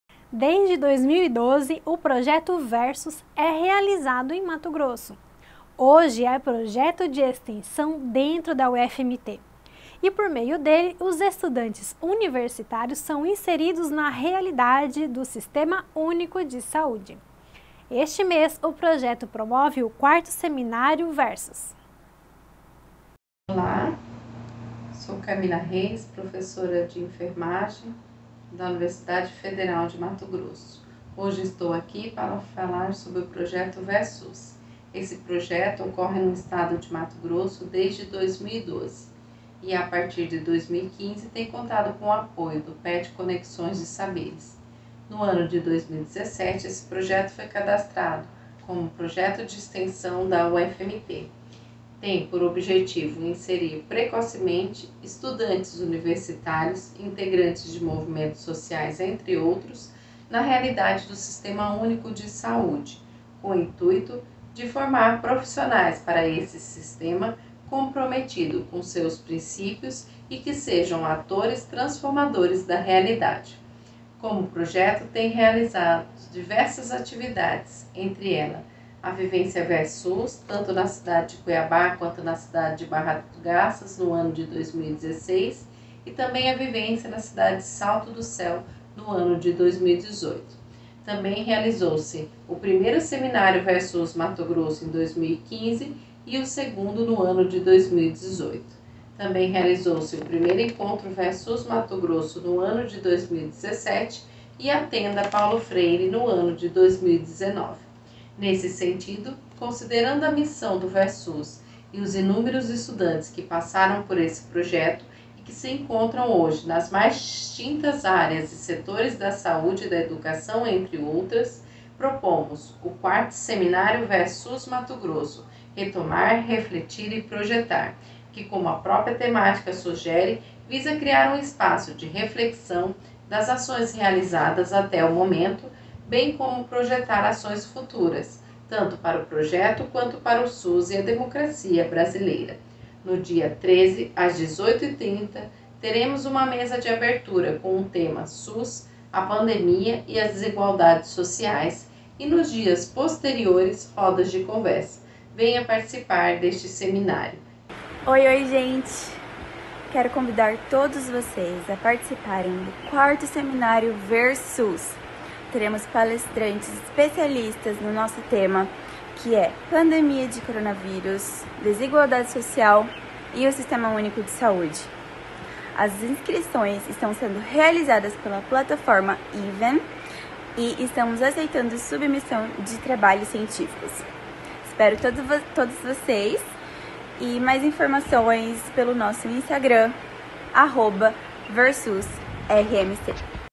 IV Seminário Ver-SUS ocorre em novembro
Desde 2012, o projeto Versus é realizado em Mato Grosso.

E hoje é um projeto de extensão dentro da UFMT, pelo qual os estudantes universitários são inseridos na realidade do sistema único de saúde.

Neste mês de novembro, o projeto promove o quarto Seminário Versus.